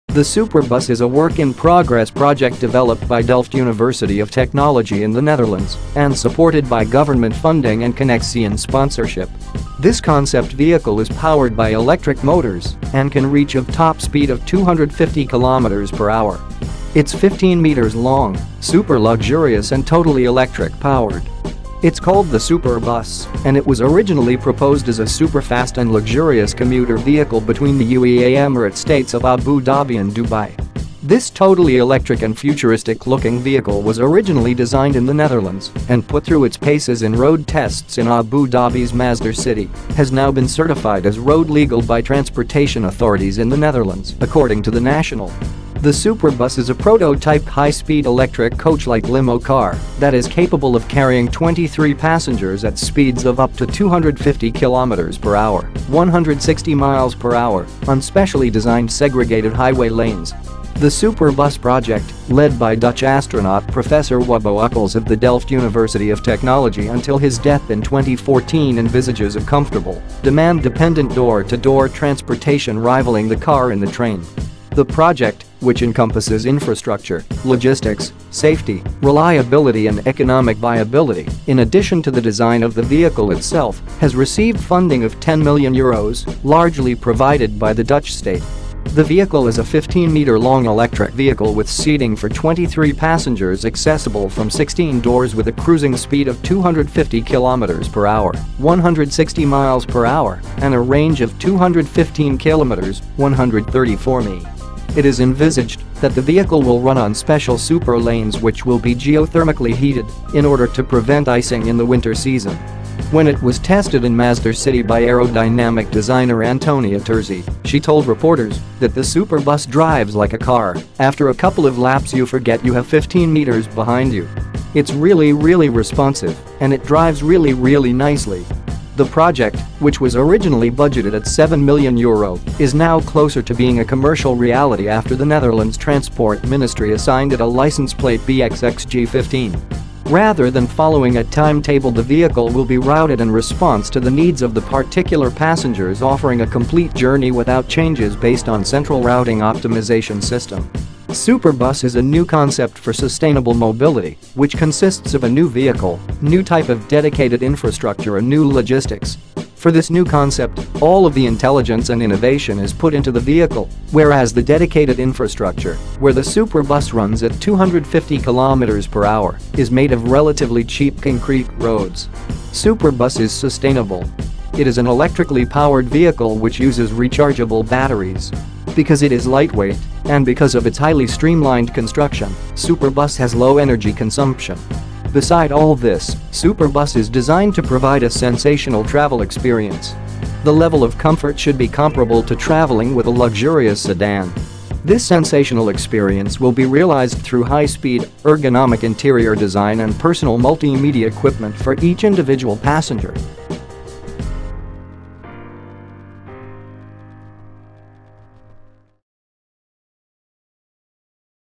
Superbus: Dubai Electric Superbus
The Superbus is a work in progress project developed by Delft University of Technology in the Netherlands and supported by Government funding and Connexxion sponsorship. This concept vehicle is powered by electric motors and can reach a top speed of 250 km/h.
It’s 15 meters long, super luxurious and totally electric powered. It’s called the Superbus, and it was originally proposed as a super-fast and luxurious commuter vehicle between the UEA Emirate states of Abu Dhabi and Dubai.  This totally electric and futuristic looking vehicle was originally designed in the Netherlands and put through its paces in road tests in Abu Dhabi’s Masdar City, has now been certified as “road Legal” by transportation authorities in the Netherlands, according to The National.
The Superbus is a prototype high speed electric coach-like limo car that is capable of carrying 23 passengers at speeds of up to 250 kilometres per hour (160 mph) on specially designed segregated highway lanes. The Superbus project, led by Dutch astronaut professor Wubbo Ockels of the Delft University of Technology until his death in 2014 envisages a comfortable, demand-dependent door-to-door transportation rivaling the car and the train. The project, which encompasses infrastructure, logistics, safety, reliability and economic viability, in addition to the design of the vehicle itself, has received funding of €10 million, largely provided by the Dutch State.
The vehicle is a 15-meter-long electric vehicle with seating for 23 passengers accessible from 16 doors with a cruising speed of 250 kilometres per hour (160 mph) and a range of 215 kilometres (134 mi).
It is envisaged that the vehicle will run on special 'Super Lanes' which will be geothermically heated in order to prevent icing in the winter season. When it was tested in Masdar City by aerodynamic designer Antonia Terzi, she told reporters that the Superbus drives like a car: “After a couple of laps you forget you have 15 metres behind you. It’s really, really responsive and it drives really, really nicely.”
The project, which was originally budgeted at 7 million Euro, is now closer to being a commercial reality after the Netherlands transport ministry assigned it a license plate BX-XG-15.
Rather than following a timetable the vehicle will be routed in response to the needs of the particular passengers offering a complete journey without changes based on central routing optimization system.
Superbus is a new concept for sustainable mobility, which consists of a new vehicle, new type of dedicated infrastructure and new logistics. For this new concept, all of the intelligence and innovation is put into the vehicle, whereas the dedicated infrastructure, where the Superbus runs at 250 km/h, is made of relatively cheap concrete roads.
Superbus is sustainable. It is an electrically powered vehicle which uses rechargeable batteries. Because it is lightweight and because of its highly streamlined construction, Superbus has low energy consumption.
Beside all this, Superbus is designed to provide a sensational travel experience. The level of comfort should be comparable to travelling with a luxurious sedan. This sensational experience will be realized through high speed, ergonomic interior design and personal multimedia equipment for each individual passenger.
Superbus
superbus dubai
electric bus
china bus
big bus
superbus dubai
Electric bus
abu dhabi
china bus
big bus
wubbo ockels